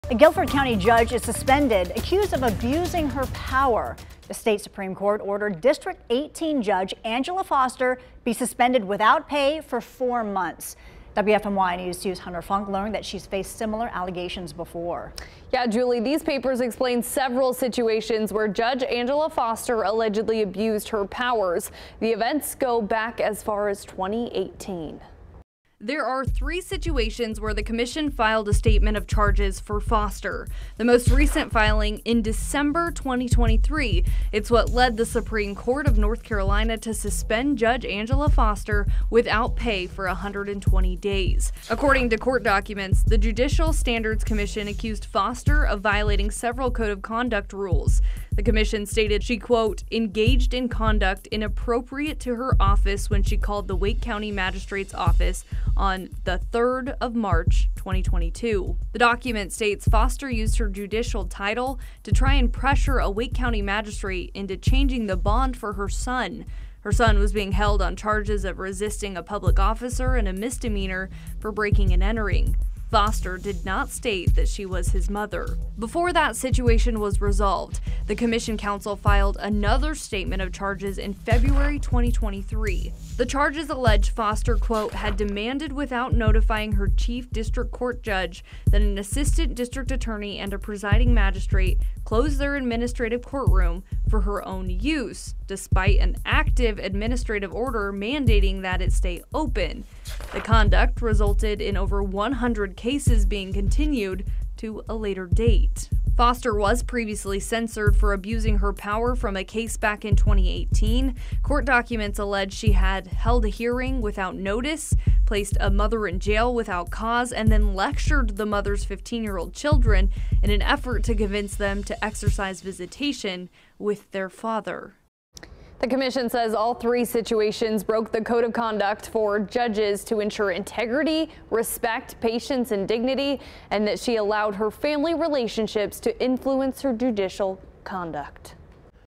Guilford County judge accused of abuse of power
She’s been suspended without pay. This is what law enforcement accuses her of.